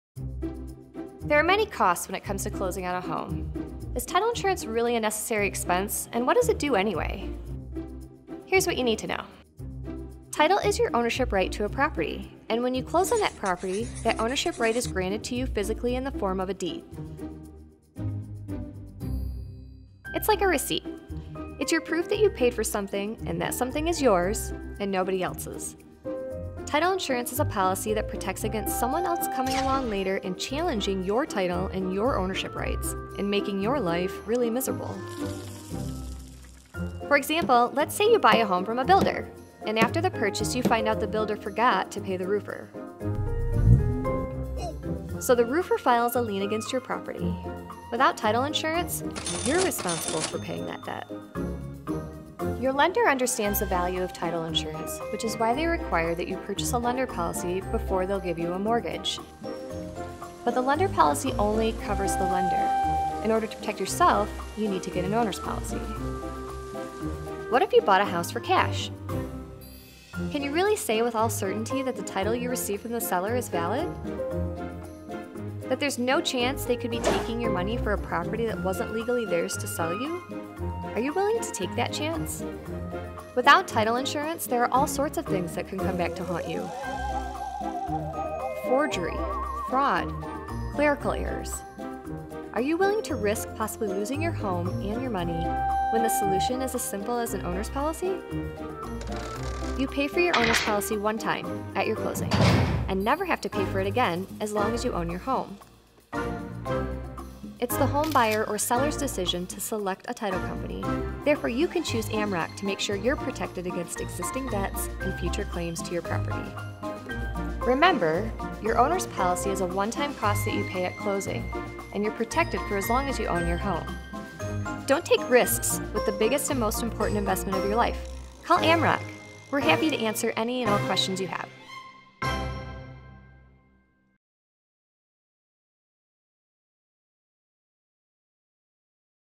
What is Title Insurance?
What is Title Insurance and why it matters? Your benefits to title insurance as well!
All of these answers come to you thanks to Amrock, a leading national provider of Title Insurance and a force in the FinTech industry.

There are two different types of title insurance: the lender’s policy and the owner’s policy. The buyer pays for the lender’s policy. It’s required  by the lender and it protects  their interest. Depending on state  and local customs, the owner’s policy may be paid for by the buyer or  the seller. An owner’s title insurance policy is not mandatory, but as a buyer, it’s the best way to protect your property rights.

Sometimes title issues can come  up after the title search. They may include forgery, fraud or clerical errors.  Your owner’s title insurance policy  lasts as long as you or your heirs own your property. It’s the best way to protect your property from future  legal claims.

Your benefits
Every year, the vast majority of home buyers in America elect to protect their investment by purchasing owner’s title insurance. The one-time payment for owner’s title insurance is low relative to the value of  your home.